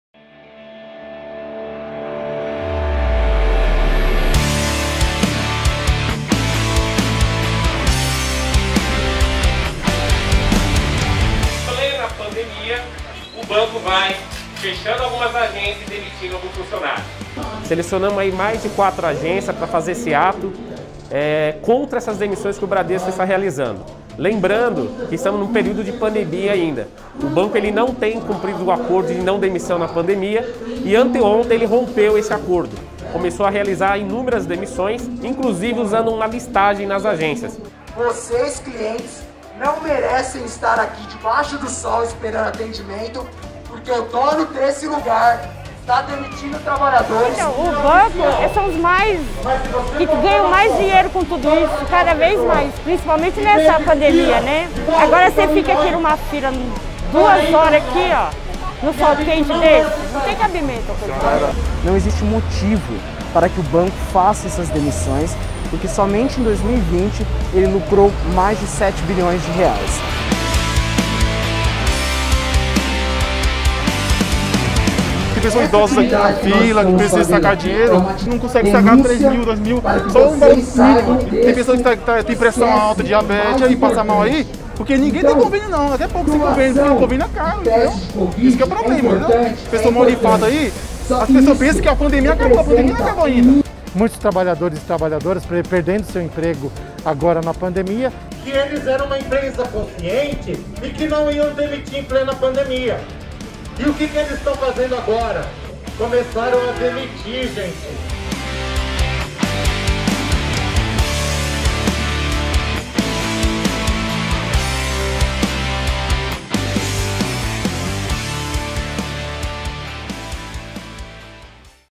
Bancários iniciam protestos contra demissões no Bradesco durante a pandemia
Os bancários reagiram e protestaram, nesta sexta 2, contra o fechamento de 500 agências do Bradesco no país e as cerca de 1,3 mil demissões que ocorreram durante a pandemia.

| Estamos construindo um jornalismo financiado exclusivamente pelo povo. Faça parte desta evolução